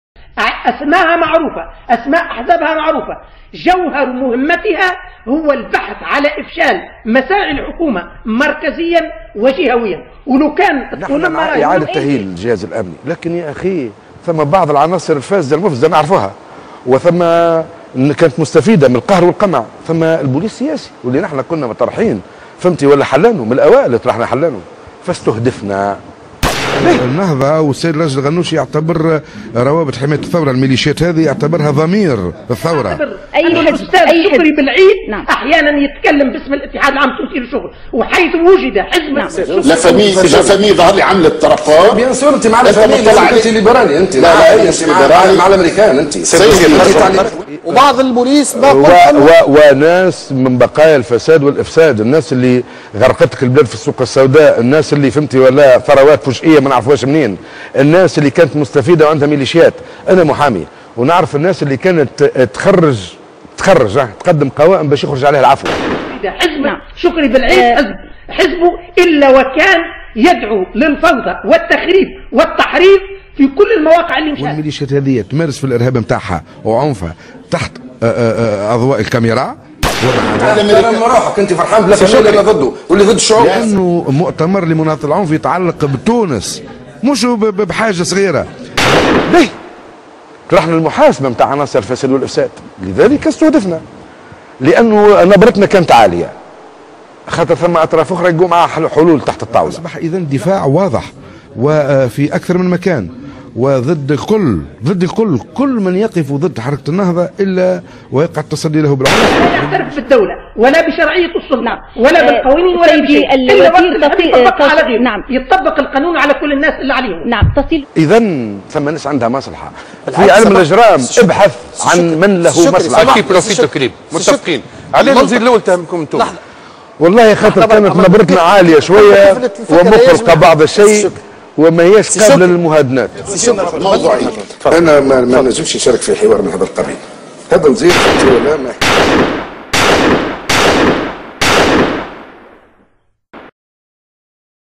Chokri Belaïd: Rest In Peace
Un des leaders de la gauche tunisienne liquidé devant chez lui, le 6/02/13.
Paix à son ame.